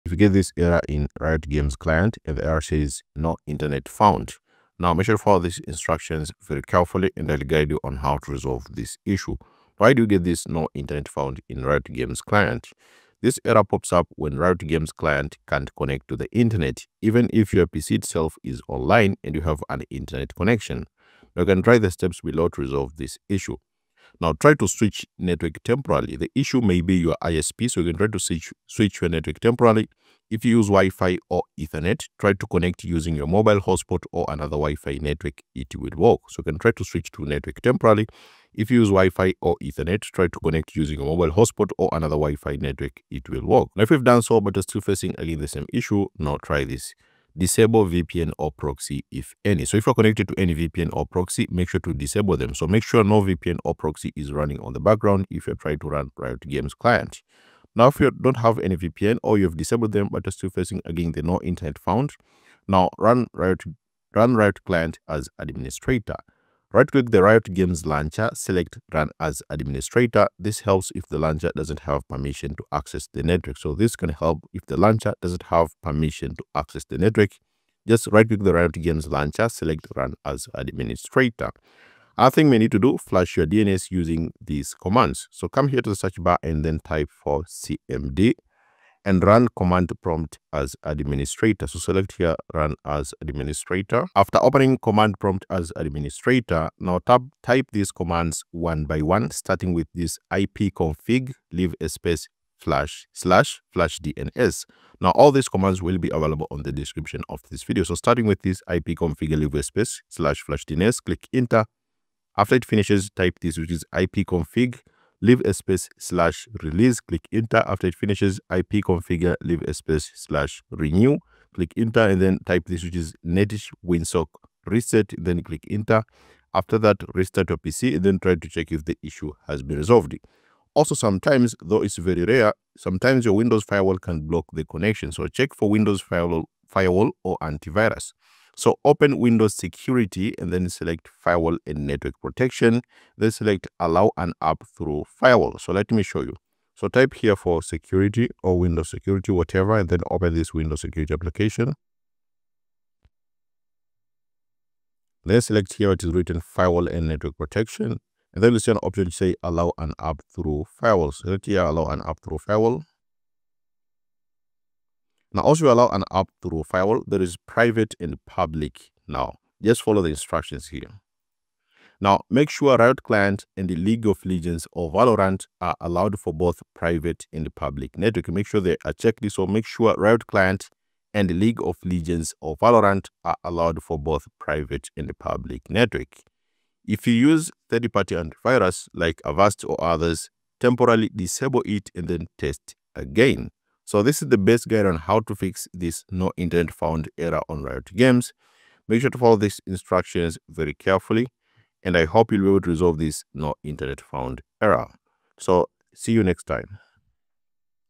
No internet found Riot Games (Quick fix)
If you get No internet found, Error When Downloading or updating RIOT Games now, this error pops up when the Riot Games client can’t connect to the internet, even if your PC itself is online and you have an internet connection.

To fix this issue follow the steps below

Switch Network Temporarily
If you use WI-FI or Ethernet, try to connect using your mobile Hotspot or another wi-fi network. It will work

Disable VPN or Proxy (if any)
Make sure no VPN or proxy is running.

Run Riot Client as Administrator
Right-click the Riot Games launcher.
Select Run as Administrator.
This helps if the launcher doesn’t have permission to access the network.

Flush Your DNS Using this Commands
ipconfig /flushdns
ipconfig /release
ipconfig /renew
netsh winsock reset
Then restart your PC.

Check Windows Firewall / Antivirus
Open Windows Security → Firewall & Network Protection → Allow an app through firewall.
Make sure Riot Client and League of Legends/Valorant are allowed for both Private and Public networks.
If you use third-party antivirus, temporarily disable it and test again.